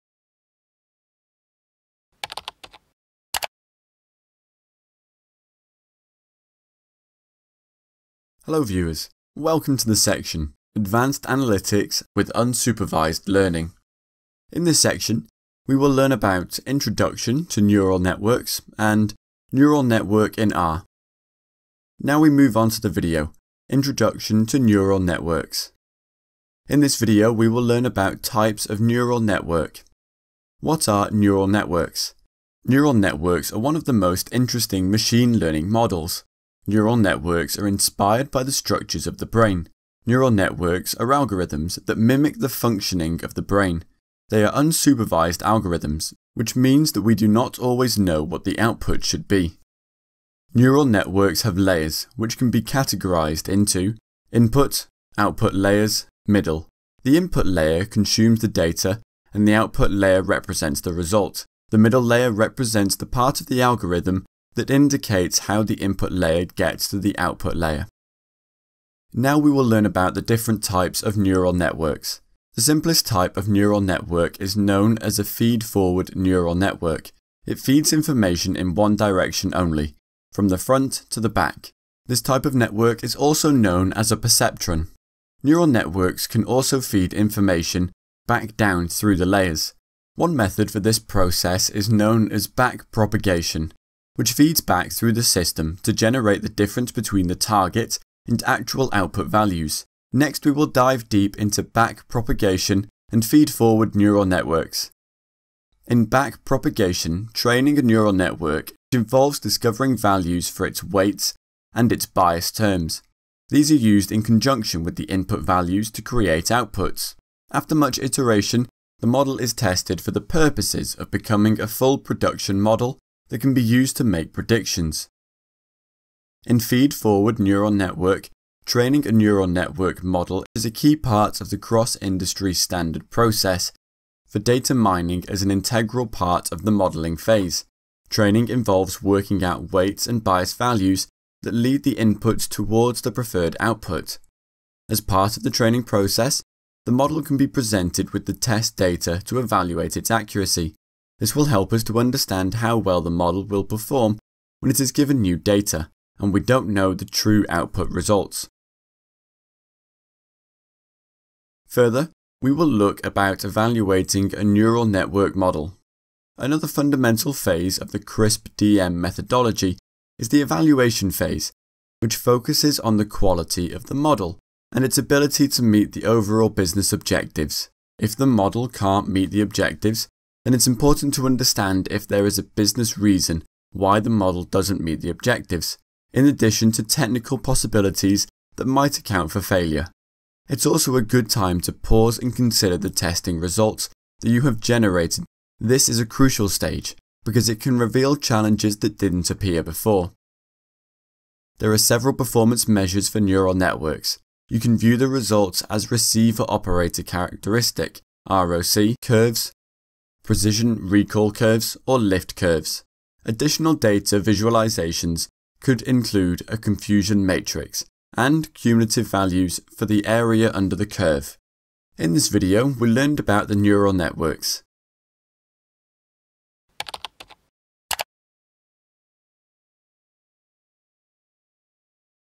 Advanced Analytics with R and Tableau : Introduction to Neural Networks | packtpub.com
Neural networks are used in business to answer these business questions. They are used to make predictions from a dataset, or to find unusual patterns. They are best used for regression or classification business problems.
• Different types of neural network